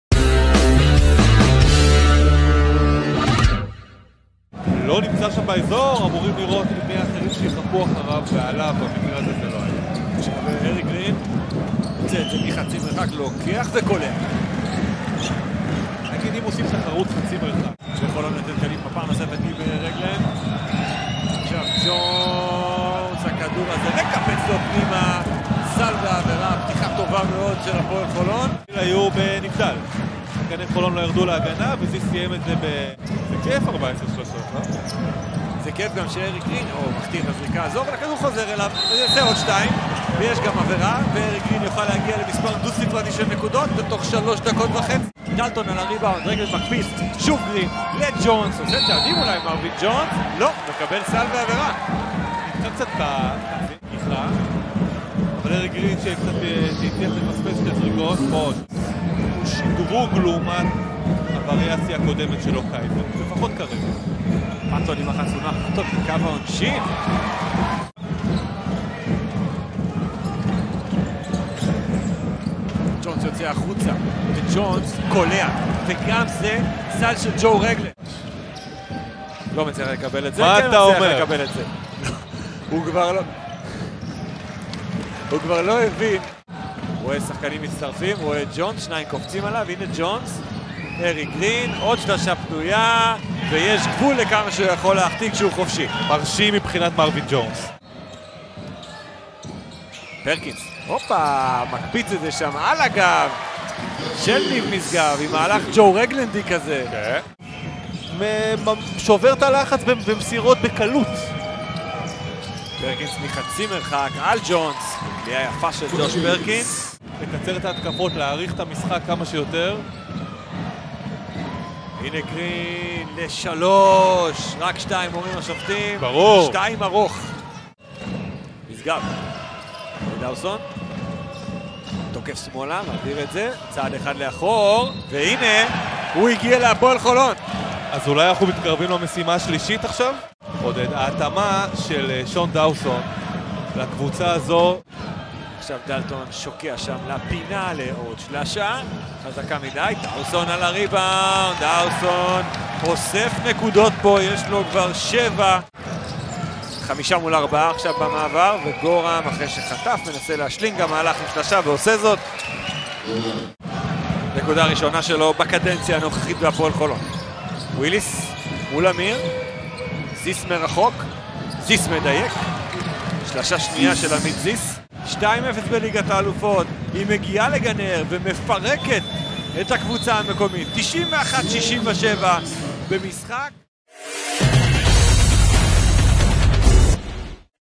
Hapoel Gilboa Galil vs. Hapoel ATSMON Holon - Game Highlights
Watch the Game Highlights from Hapoel Gilboa Galil vs. Hapoel ATSMON Holon, 10/17/2022